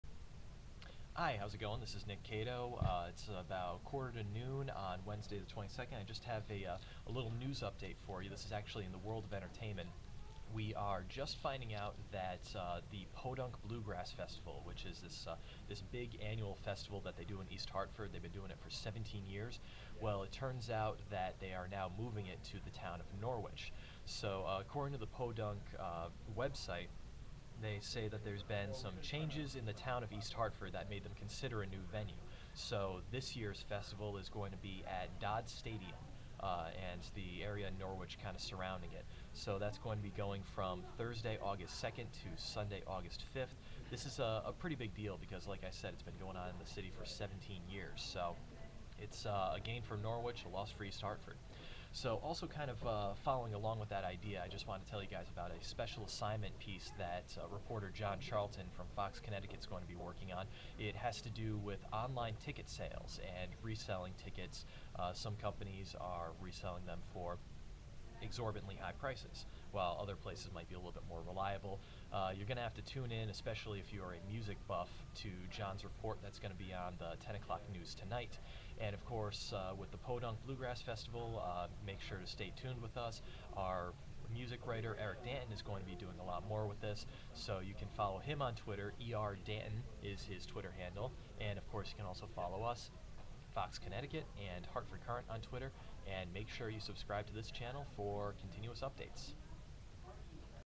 Podunk Bluegrass Fest Moving, Online Ticket Sales (11:45am 2/22)
After 17 years the Podunk Bluegrass festival is moving from East Hartford to Norwich, also John Charlton is working on a story about online ticket sales.